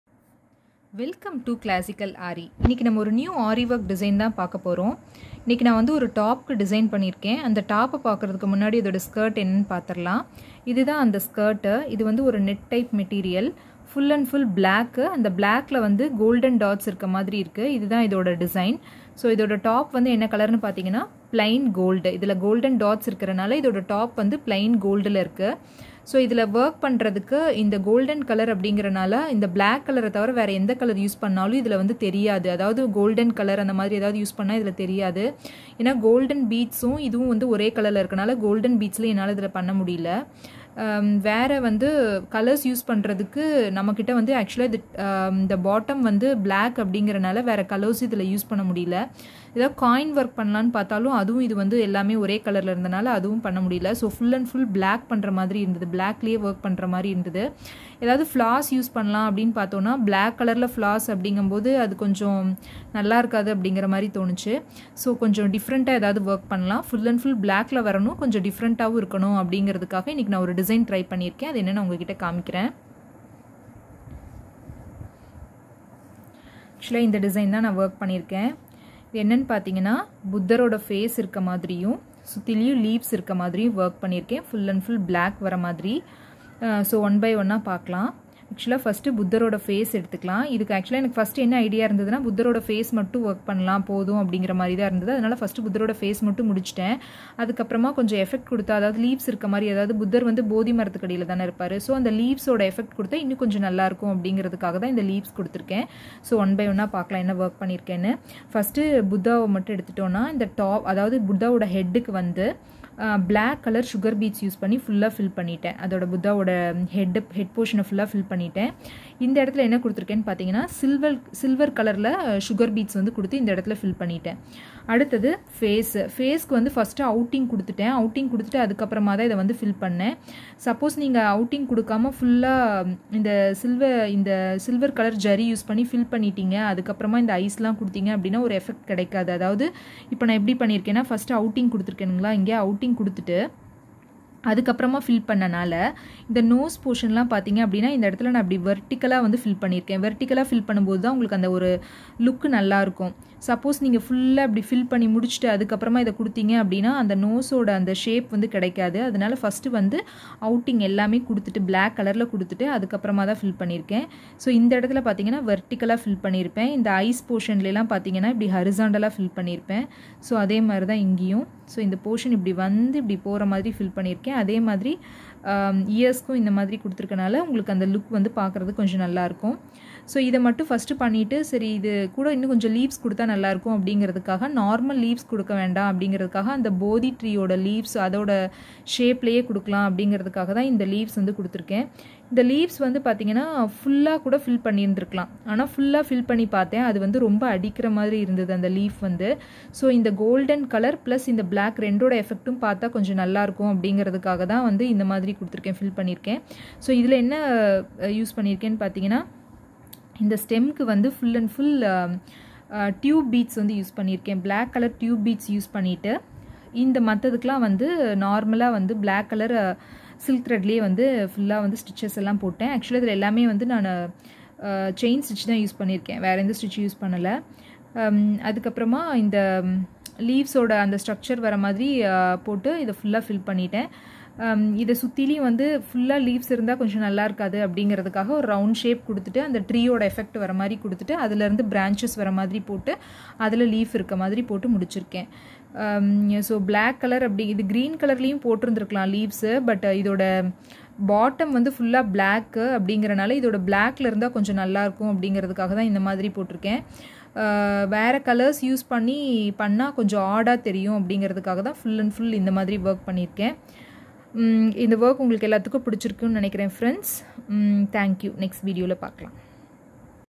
BUDDHA FACE AARI WORK DESIGN FOR TOPS IN TAMIL
Buddha face aari work design in tops.This design suits for blouse ,kurta top.